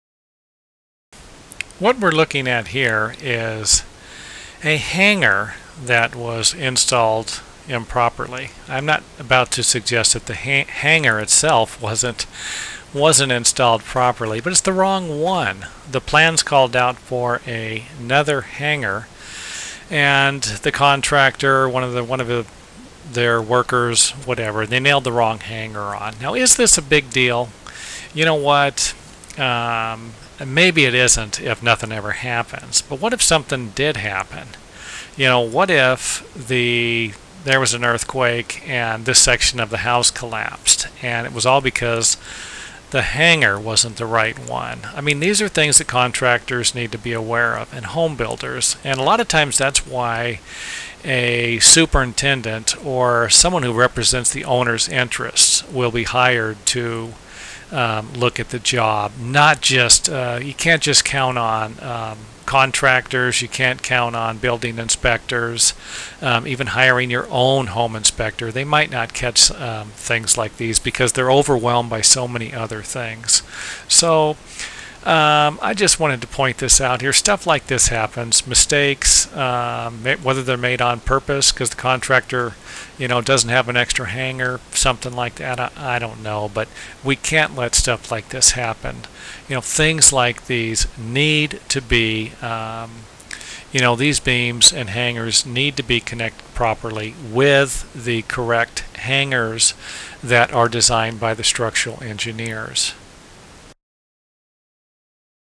Structural Framing Problems - Wrong Beam Hanger
Click on this link to see more videos on engineering, home building and repairs.  Watch this video if you're interested in learning more about some of the problems building contractors and homeowners run into, especially when it comes to wood beam hangers and structural hardware.  Don't forget to visit our other websites for more tips and ideas.